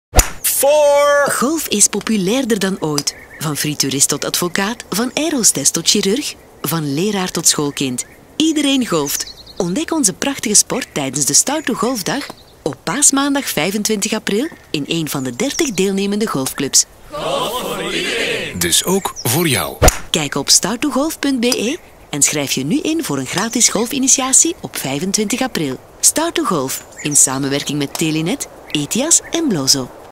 'Start to Golf' dag op paasmaandag 25 april
START TO GOLF: Golfclubs geven gratis initiatie op paasmaandag 25 april

Golf is populairder dan ooit! Van friturist tot advokaat, van airhostess tot chirurg, van leraar tot schoolkind... Iedereen golft! Ontdek deze prachtige sport tijdens de gratis Start to Golf dag op paasmaandag 25 april in één van de 30 deelnemende golfclubs.

"Meer dan 33.000 gepassioneerde mensen beoefenen onze sport. Dat is meer dan het dubbele ten opzichte van 15 jaar geleden. De ambitie van de Vlaamse Vereniging voor Golf (VVG) is duidelijk. We willen tegen 2020 het aantal golfspelers optrekken tot 50.000. Golf is een sport voor jong en oud, voor sportief en minder sportief, voor mensen die alleen of met het hele gezin van de natuur willen genieten. Het is een sport voor mensen met veel of weinig vrije tijd die op zoek zijn naar een aangename sportieve sfeer en die gezond willen bewegen", zegt de Vlaamse Vereniging voor Golf.

Start to Golf is dé ideale manier om met golf te beginnen. Start to Golf, alsook het Start to Golf-boek in samenwerking met Evy Gruyaert, beschrijft in een lessenreeks het golfspel en hoe je het best met golf kan beginnen.  Een stage bestaat uit een aantal lessen waarbij de huur van de infrastructuur, de lesgever en golfmateriaal zijn inbegrepen. Er zijn reeds lessenreeksen vanaf 100 euro.

Alle info over 25 april en de deelnemende golfclubs op Om zeker te zijn van een plekje tijdens de Start to Golf dag is voorinschrijving belangrijk!

Start to Golf, in samenwerking met Telenet, Ethias en Bloso én met mediasteun van Trends, Metro, Sportmagazine, Het Belang van Limburg, Gazet van Antwerpen, Elle België, Radio Nostalgie, Kanaal Z, Acht, Sporza en alle regionale zenders.